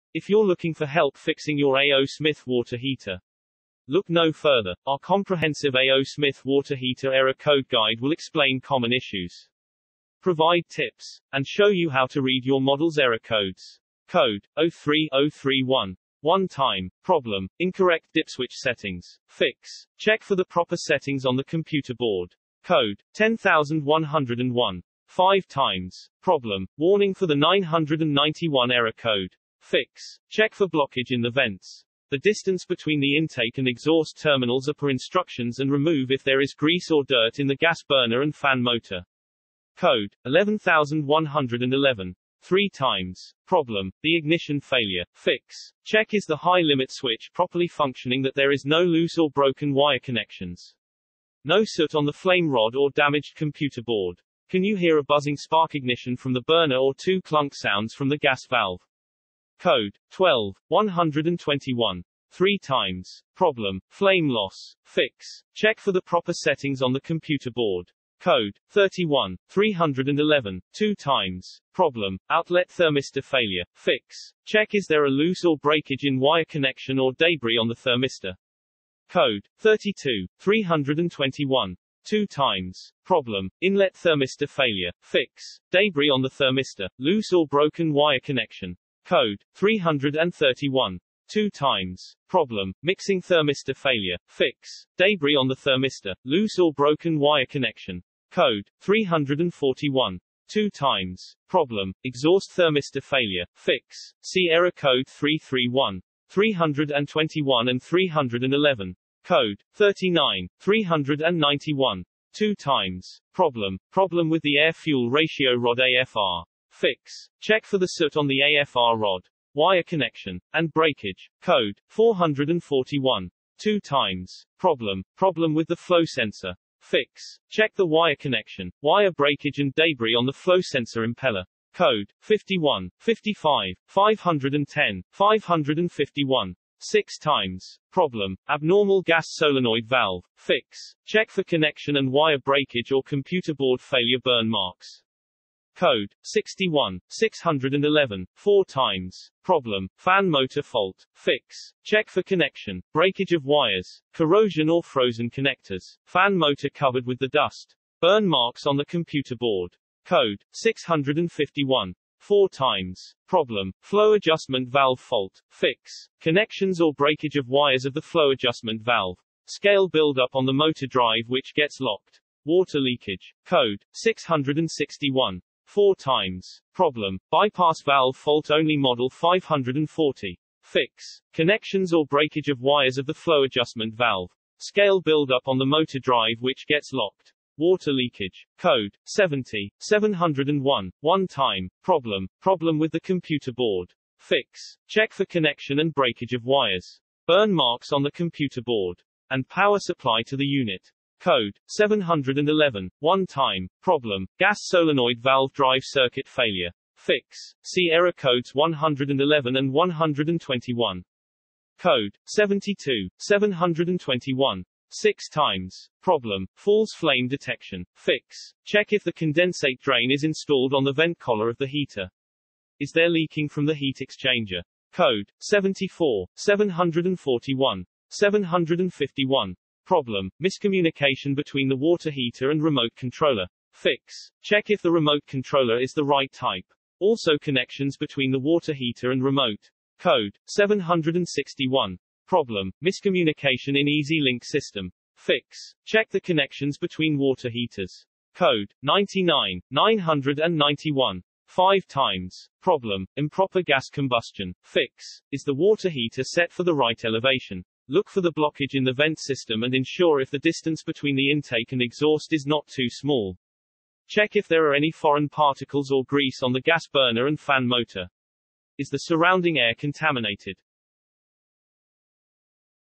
A.O. Smith Water Heater Error Codes
If you're looking for help fixing your A.O. Smith Water Heater, look no further! Our comprehensive A.O. Smith Water Heater error code guide will explain common issues, provide tips, and show you how to read your model's error codes.